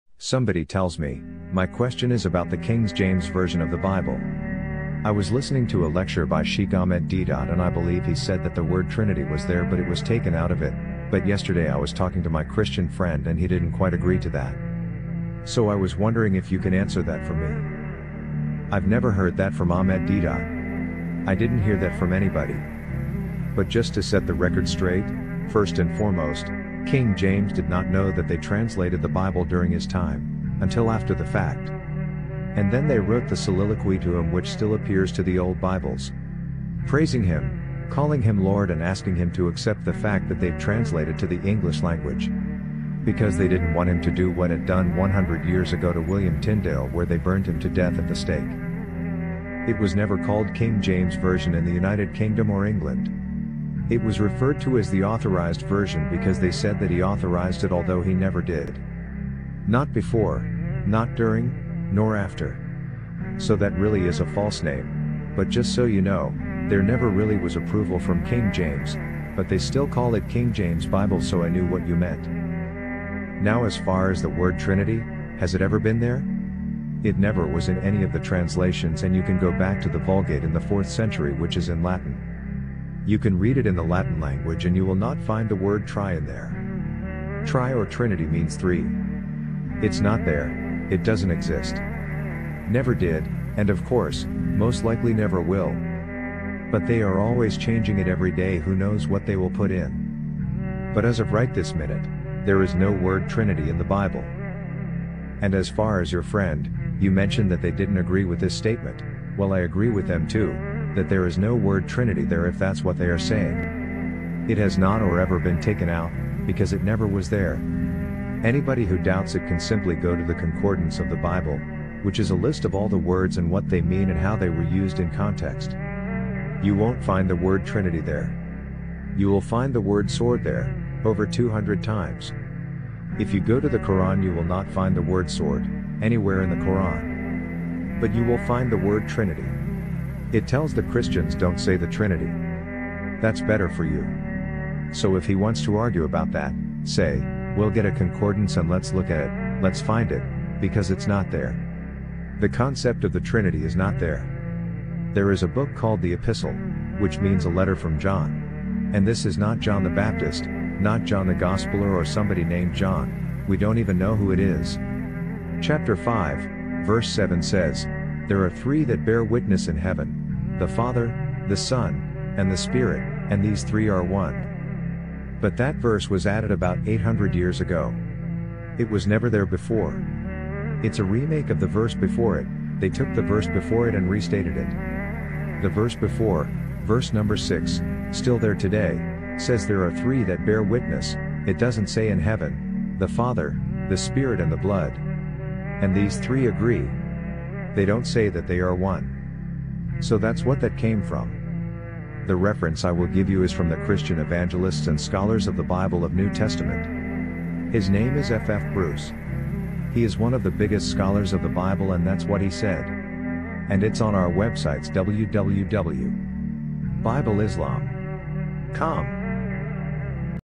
Is The Word Trinity in The Bible
Islamcompass.com

[Noble Quran 7:54]

 "Indeed, your Lord is Allah, Who created the heavens and the earth in six yaum (days or periods of time), and then He Istawa (rose above), the Throne, disposing of the affair of everything. No intercessor (can plead with Him) except by His Permission. This is Allah, your Lord, so worship Him. Won't you then remember?" [Noble Quran 10:3]

 He is the One Who created the heavens and the earth in six yaum (days or periods of time), and then He Istawa (rose above) the Throne. He knows what goes into the world and what comes forth from it, what descends from heaven, and what ascends up to it. And He is with you (by His Knowledge) where so ever you may be. And Allah is the All-Seer of what you do. [Noble Quran 57:4]

We observe from these statements a very logical approach to understanding the nature of God without comparing Him to the creation or putting Him in His creation. Is The Word Trinity in The Bible Is The Word Trinity in The Bible